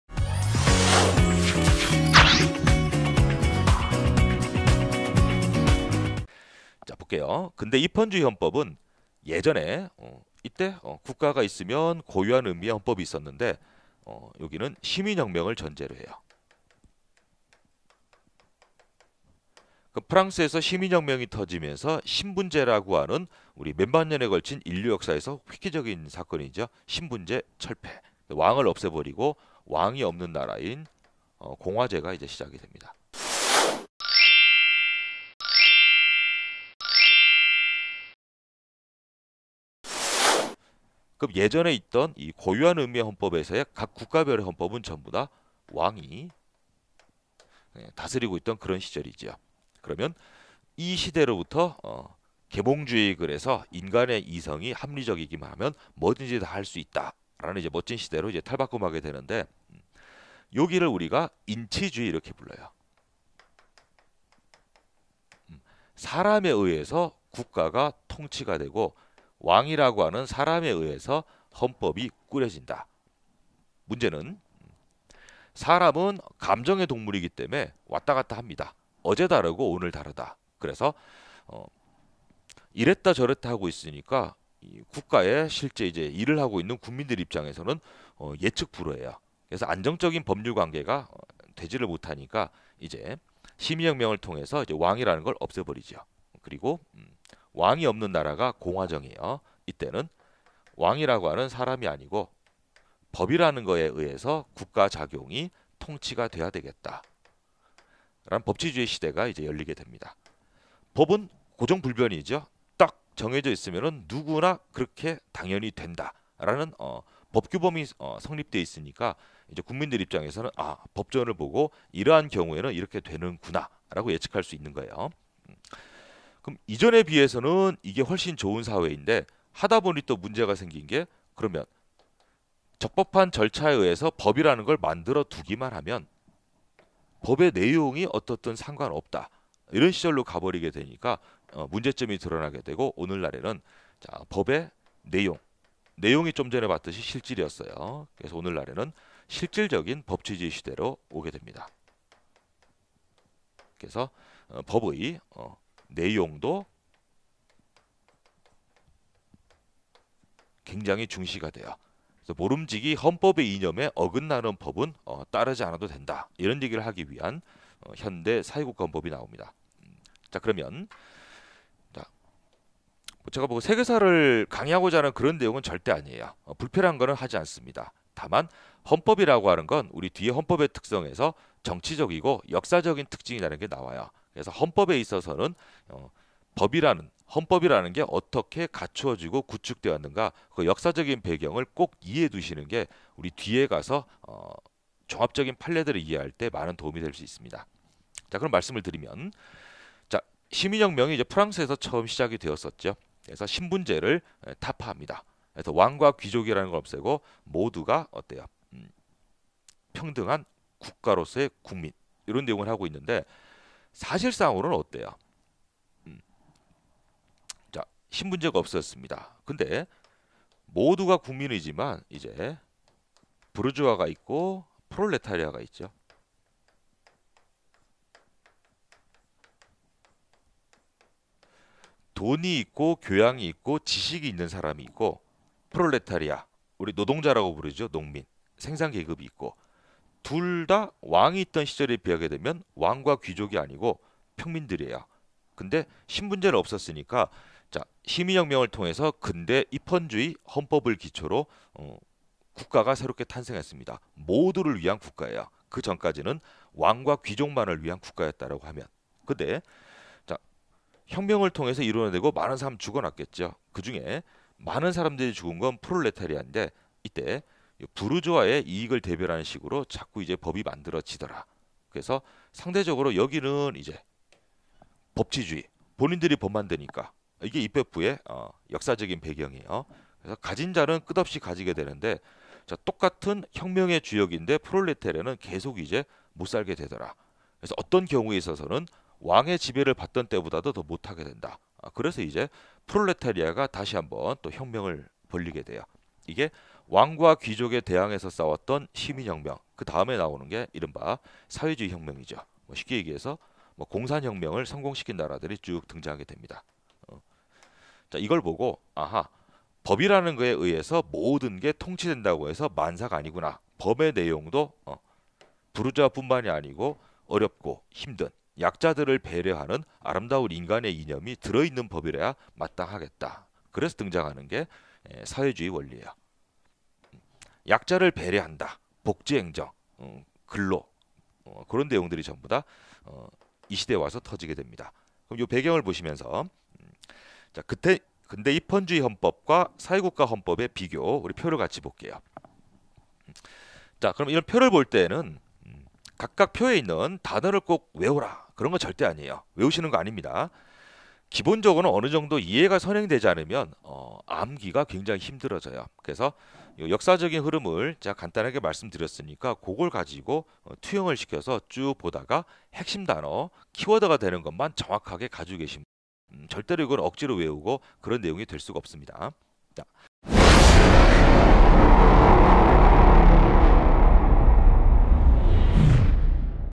경찰직 헌법 기본과정 - 헌법의 개념 3장
헌법의 발전과정에 따른 개념
1. 공유한 의미의 헌법 = 국가
2. 근대 입헌주의 헌법 = 시민혁명
3. 현대 사회국가 헌법 = 실질적 법치주의